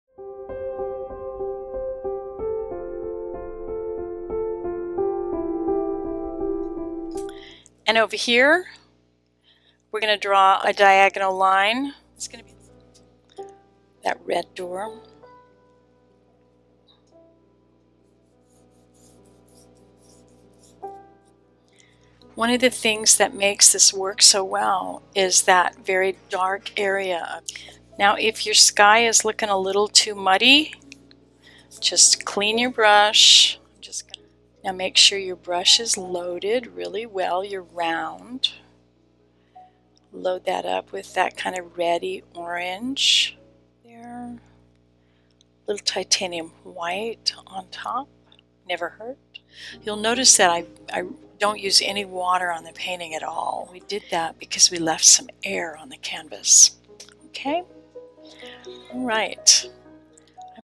Acrylic Painting Class - Henri Matisse
This is an introduction to my painting class based on Henri Matisse's painting "Open Window at Collioure."

Materials used in this class